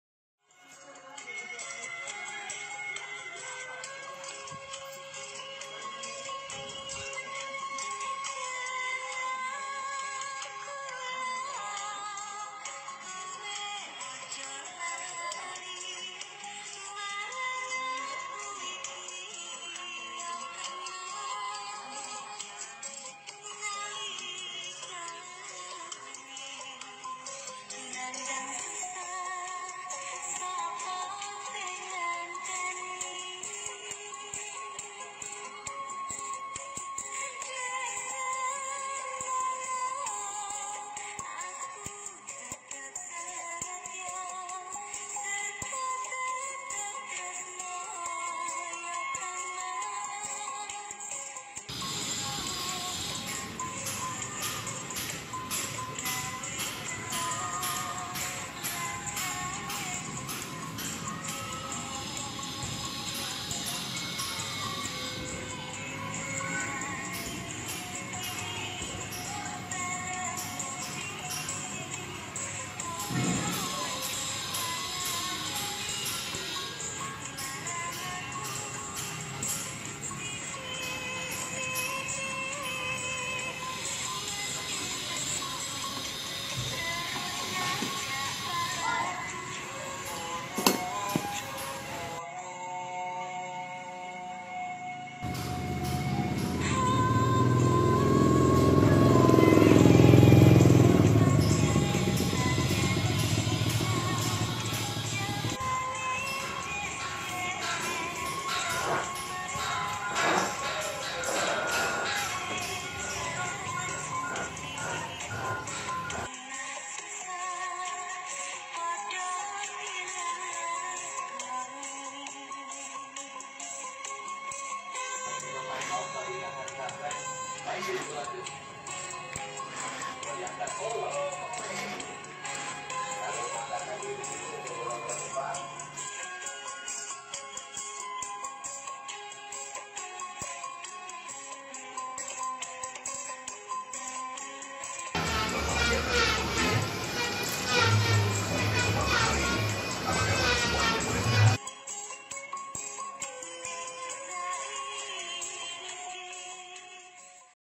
Membuat RC Sail Boat....Cargo Schooner
Hull terbuat dari fiberglass
Deck dan Cabin terbuat dari triplex 3mm
Radio Control 3 Channel